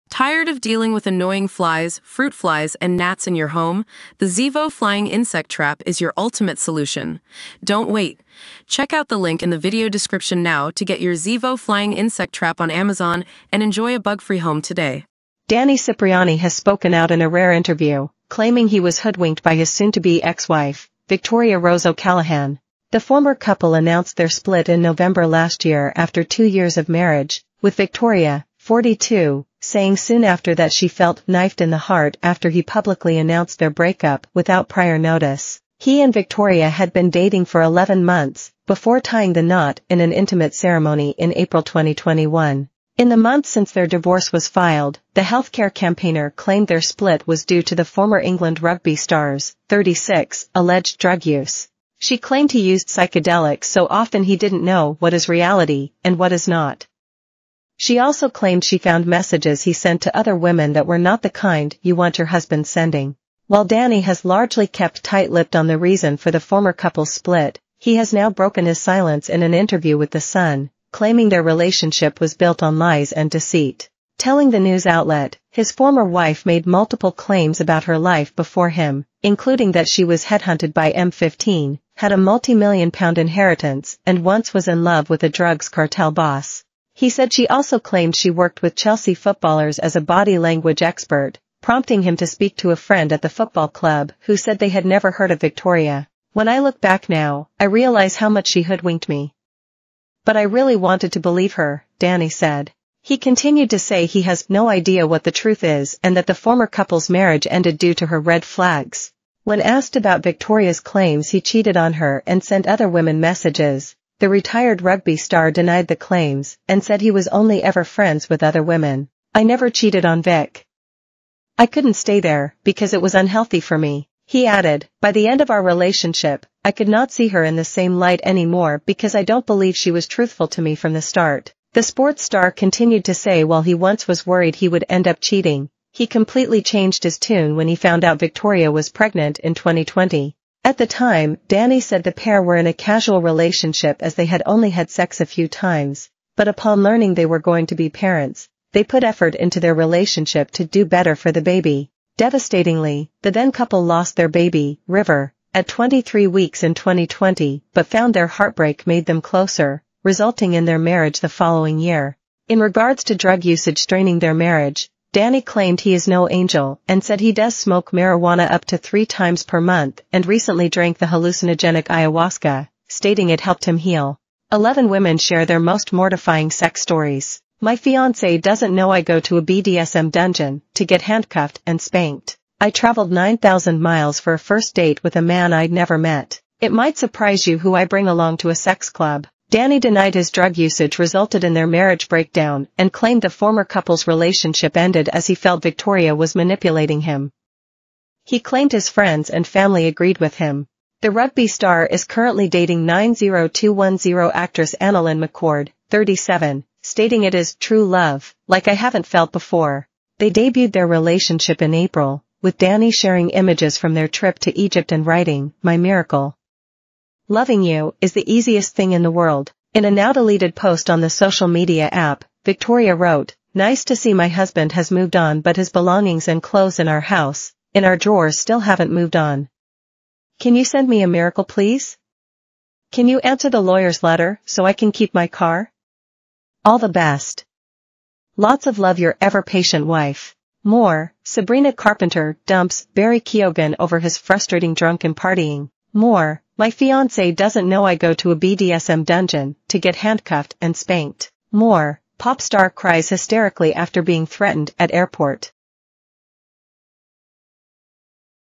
Danny Cipriani criticizes his ex-wife for building a four-year relationship based on
Danny Cipriani criticizes his ex-wife for building a four-year relationship based on “lies”